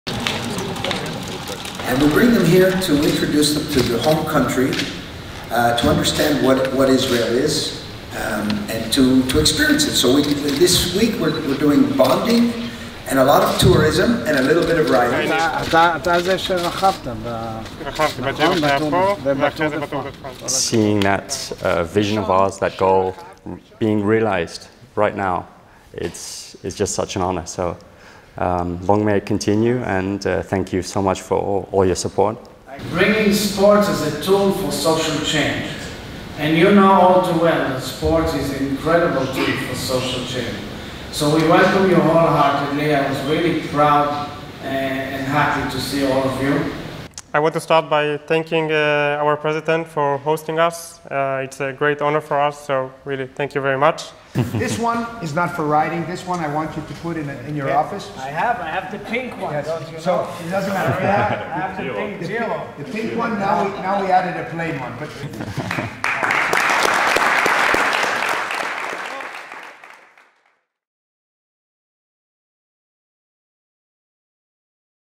Meeting the Israeli president | ISN Homecoming | Visit Israel
During our visit in Jerusalem, we met the Israeli president.

It was an honor this morning to visit Israeli president Isaac Herzog in his residential house.

He now owns two cycling jerseys: a pink Giro-jersey and a Jinga-ISN jersey!

Thank you, mister president.